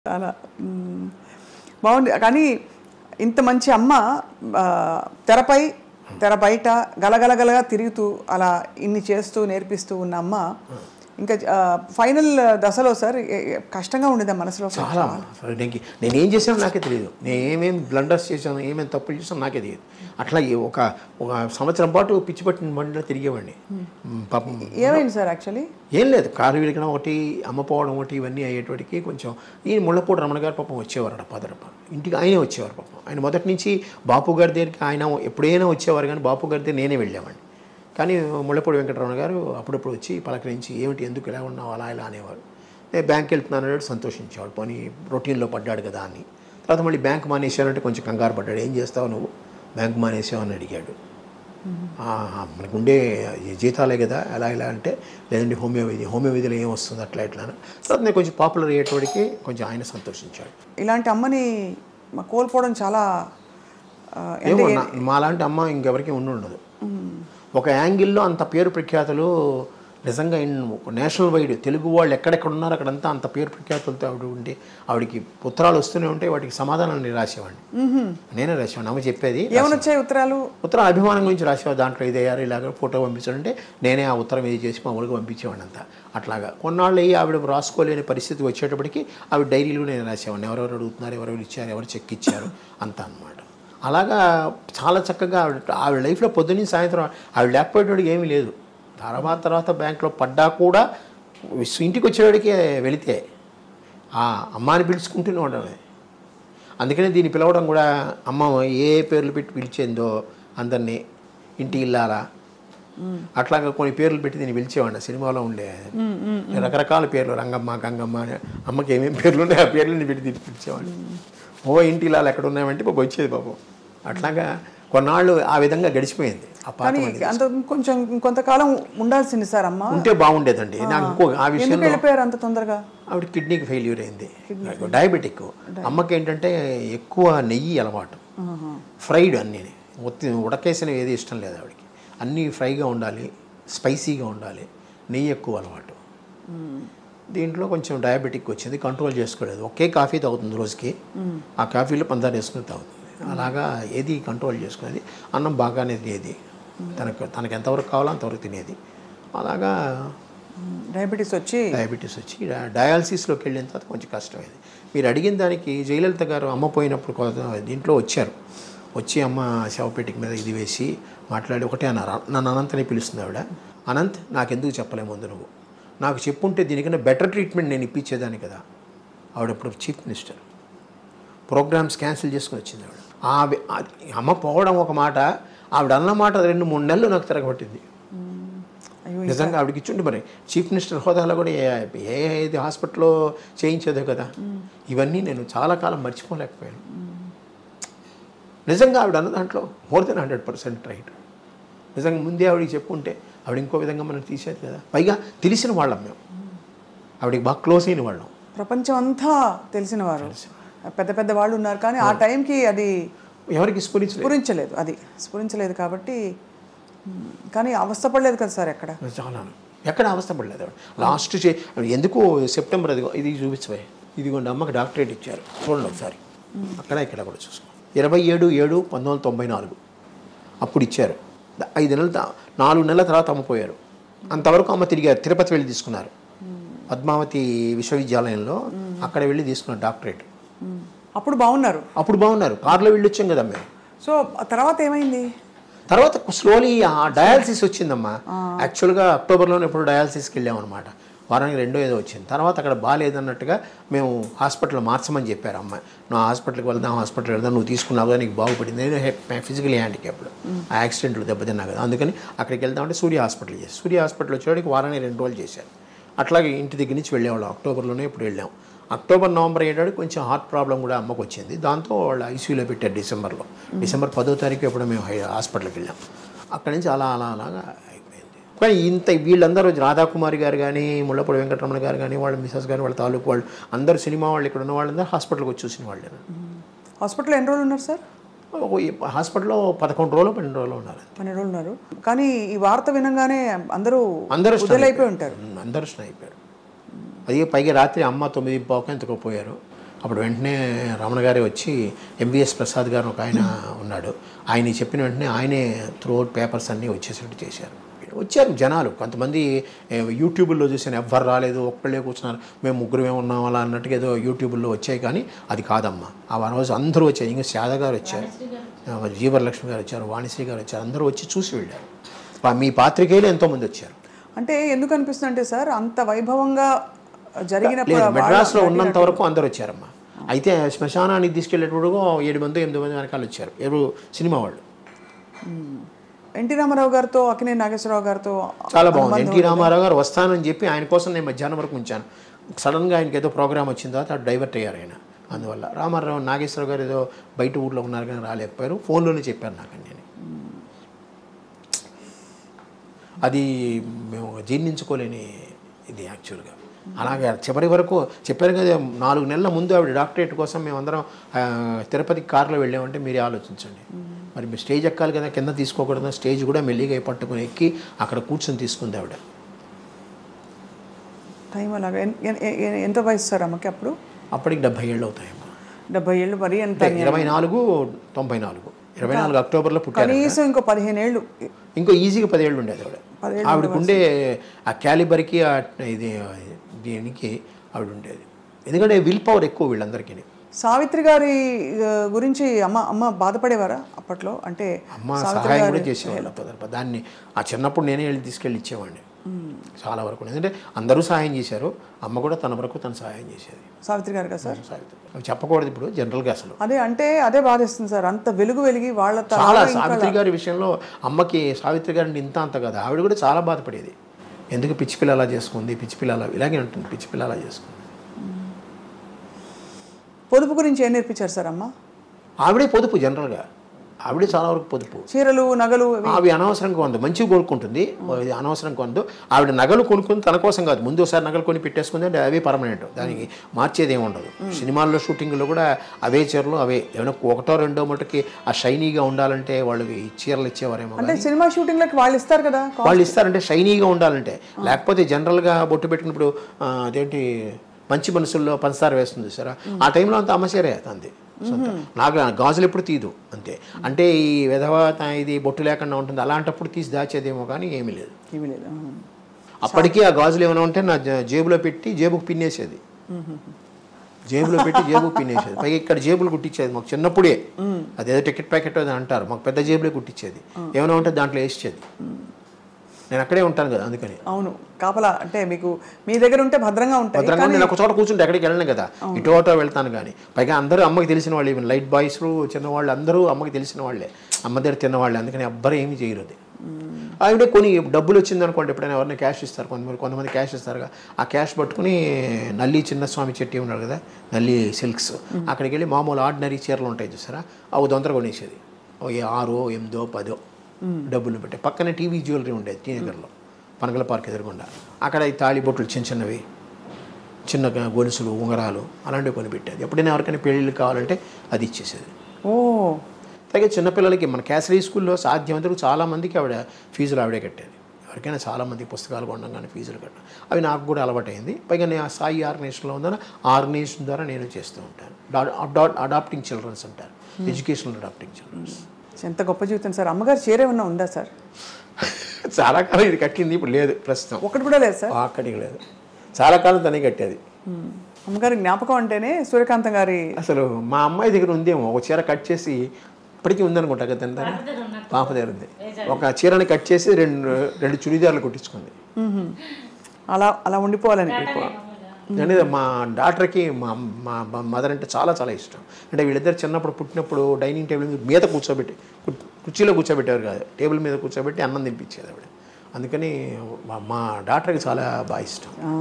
Suryakantham Son Anantha Padmanabha About His Mother Funera | Suryakantham Son Interview | iDream
Suryakantham Son Anantha Padmanabha About His Mother Funera | Suryakantham Son Interview | iDreamClips